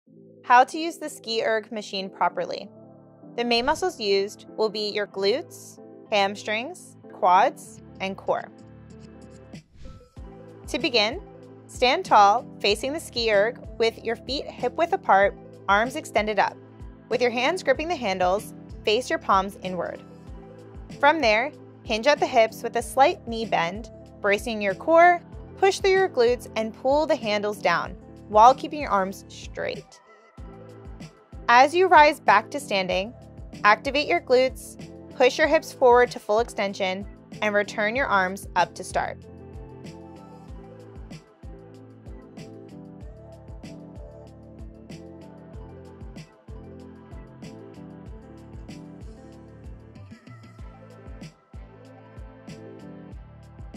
How to: Ski Erg
🏋️‍♀️How to perform the Ski Erg

Main Muscles Used:
1. Quads
2. Glutes
3. Hamstrings
4. Core

Correct Cues:

✅ Stand tall, facing the SkiErg with feet hip-width apart, arms extended up. With your hands gripping the handles, face your palms inward.

✅ Hinging at the hips, with a slight knee bend, brace the core, push through the glutes, and pull handles down. While keeping your arms straight.

✅ Continue pulling the handles down in a fluid motion until your arms swing back past your thighs.

✅ As you rise back to standing, activate your glutes, push your hips forward to full extension, and return your arms up to start.

Incorrect Cues:

❌ Squatting: Remember it's a hip hinge, like a deadlift movement, not a squat.

❌ Dropping the chest.

❌ Cutting the stroke short.

❌ Looking up.

🔥 ACHIEVE FAT LOSS FOR THE FINAL TIME ✅

🍫 Lose 30lbs eating chocolate every day!

📧 If you downloaded our masterclass and it didn't show up in your inbox, contact hello[at]bodysmartfitnessl[dot]com